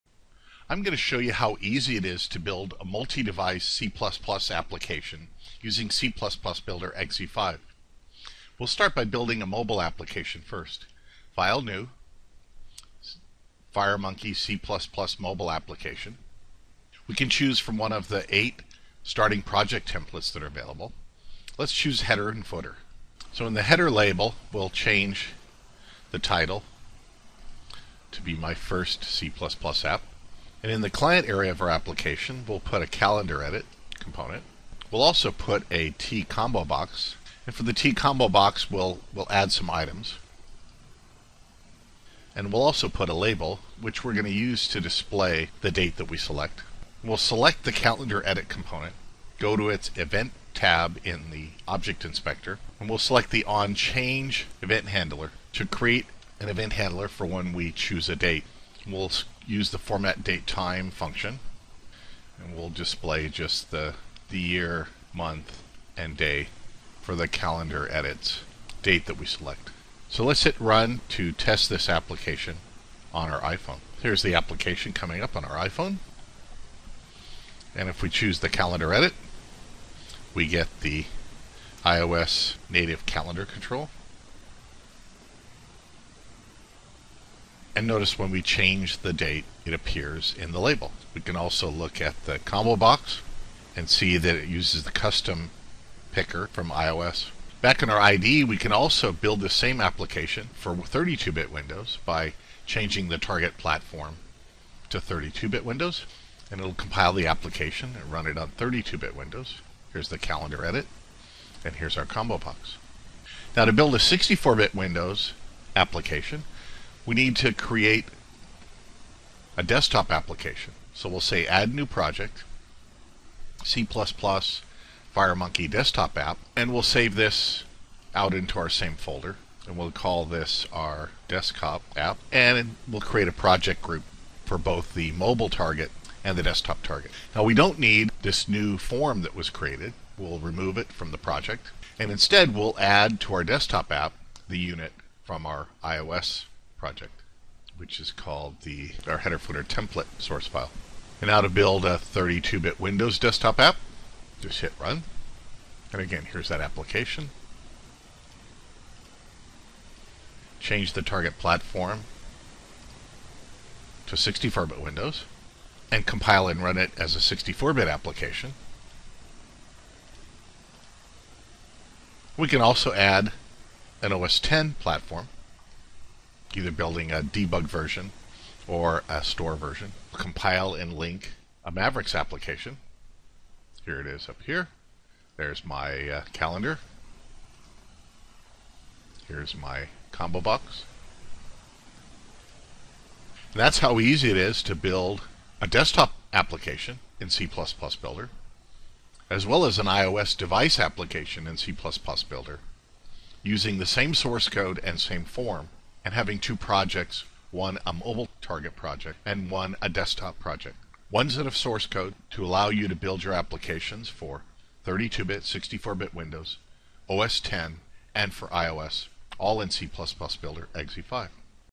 Your First C++Builder XE5 Multi-Device App for Windows (32/64), OSX and iOS
This is the first of 31 consecutive C++ for iOS videos showing you how to build your first multi-device C++Builder application for Windows (32/64), OSX and iOS.  This quick example application uses the TCalendarEdit and TComboBox components and shows how they use the native DatePicker and CustomPicker controls on iOS.  The same code and form is used for all 4 platform targets.